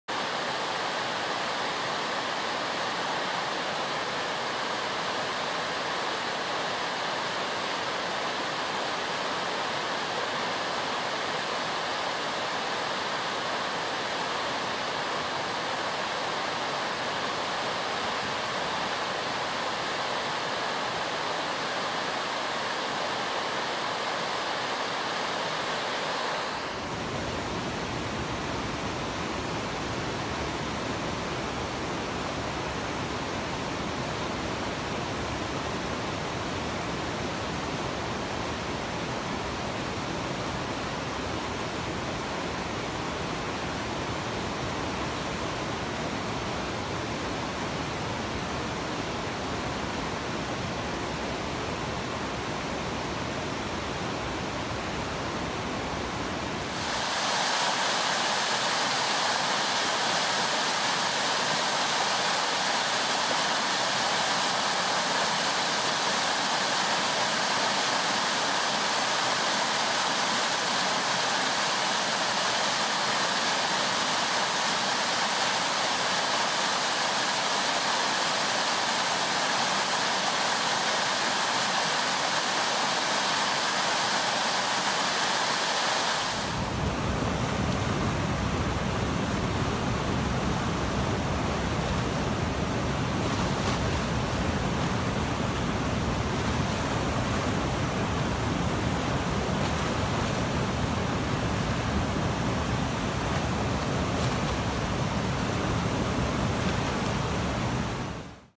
James River 5-27-17
The James River crested at 14.27 feet near Belle Isle on the morning of May 27, 2017. The first two clips are on the side of the island that is normally dry and easlily traversed from the south bank to the island. The last clip is of the Hollywood Rapids, usually an urban Class 4, looking quite angry.